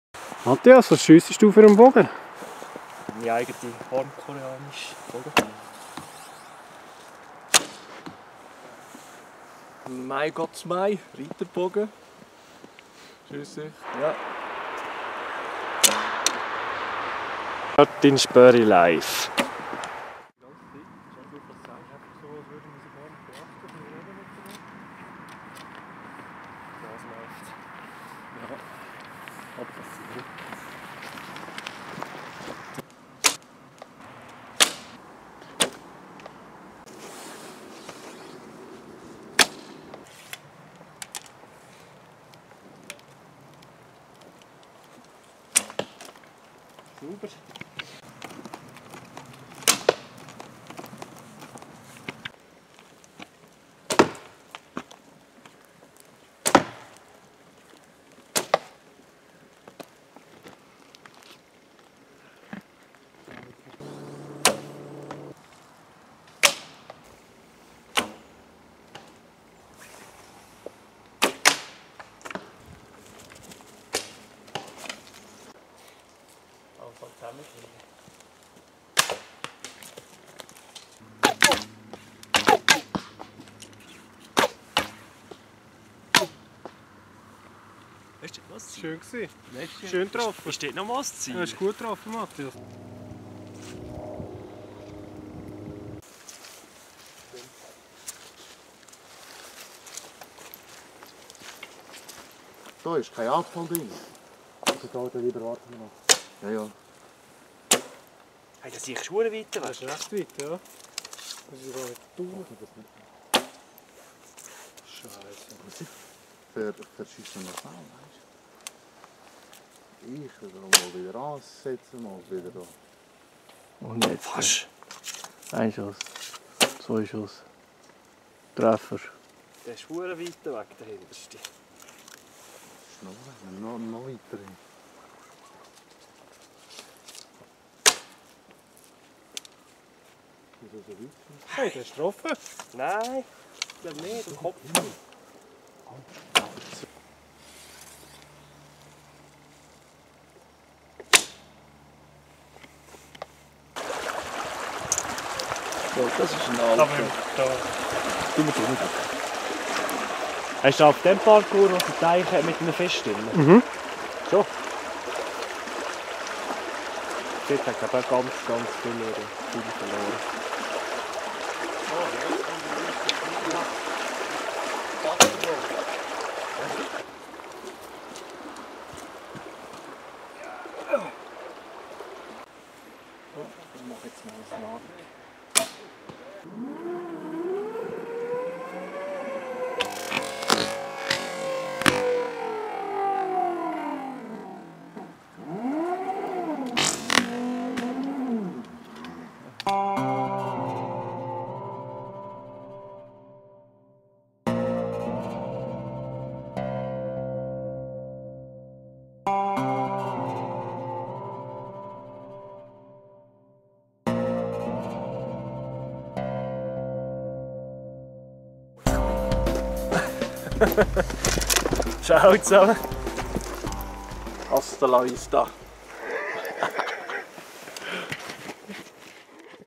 3d Fun in Donau Eschingen mit Combat Archery Switzerland
Nach monatelangem Basteln in den kalten Winternächten testen Matthias Lehner seinen neugebauten Koreaner. Karl der Grosse feilt an seinen Kurzpfeilen und Martin Spörri ist nach langer Verletzung wieder bak in Buisness

Thumb Draw Speed Shooting Arrow Tag